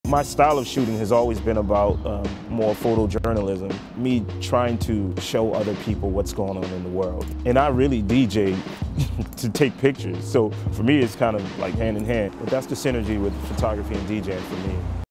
BLX SERIES SNEAK PEAK : D-NICE - BRONX, NY
D-NICE talks BDP, photography, the early days of hiphop and more in this episode of BLX (blocks).